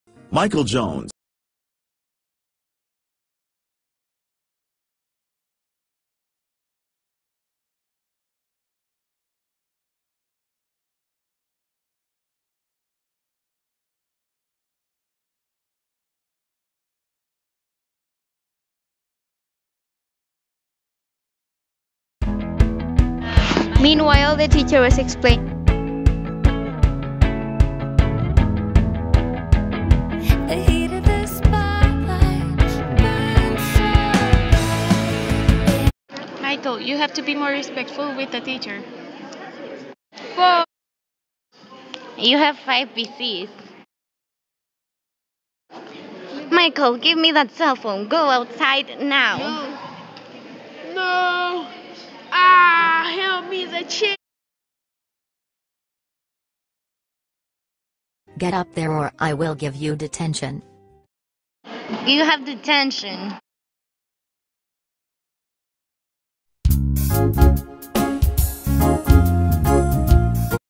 Michael Jones-Respect
A short animation about respect.